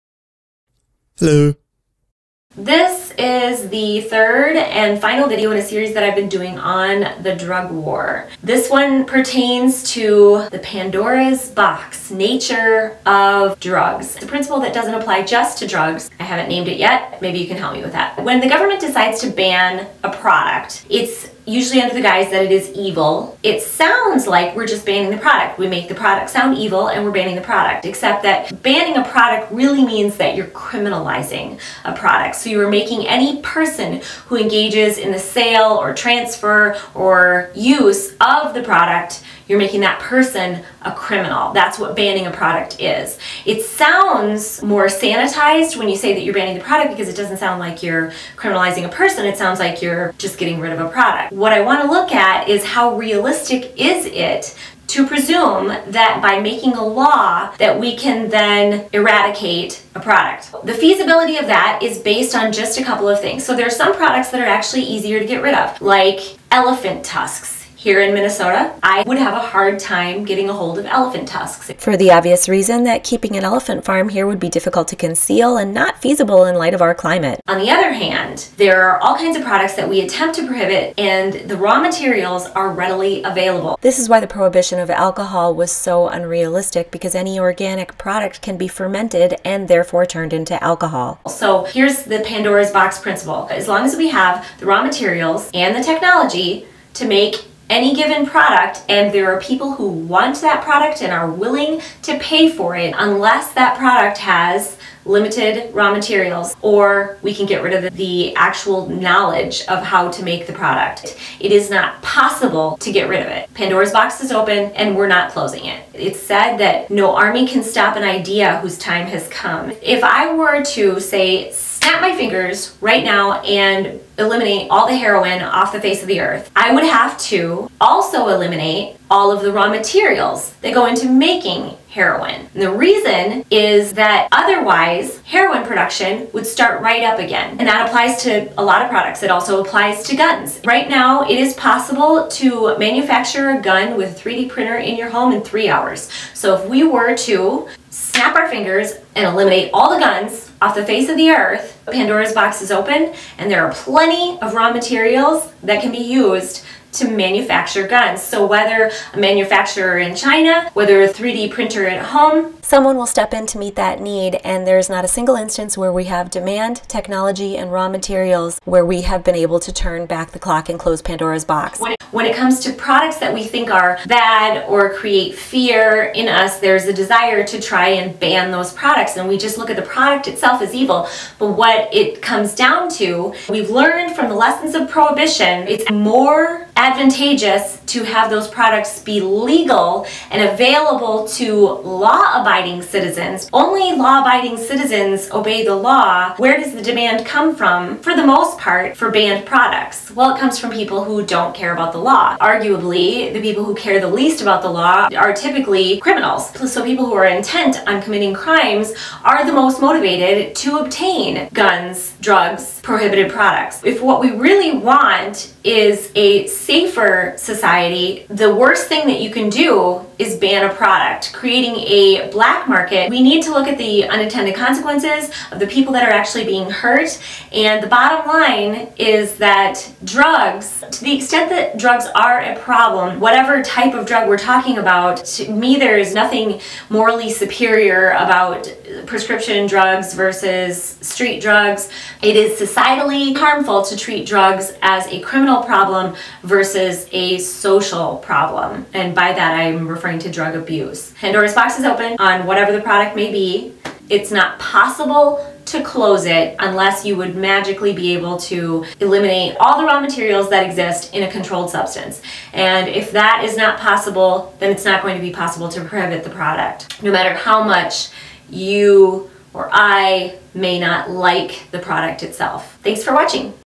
Why the War on Drugs Will Always Be A Failure: Pandora's Box
When we talk about banning products we're really talking about criminalizing them. Is it realistic to expect that the government can eradicate a product in a free society by banning it or creating a law prohibiting it? In this video I propose a simple principle that can be used to evaluable whether or not any prohibition is realistic.